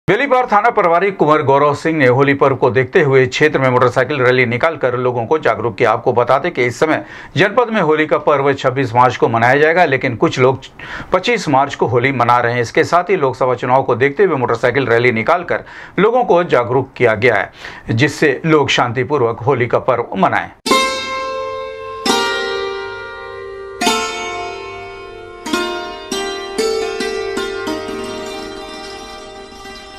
लोगो को शंत्ति पूर्व क होली मनाने का किया अपील
बेलीपार थाना प्रभारी कुँवर गौरव सिंह ने होली पर्व को देखते हुए क्षेत्र में मोटरसाइकिल रैली निकालकर लोगो को किया जागरूक,लोगो को शंत्ति पूर्व क होली मनाने का किया अपील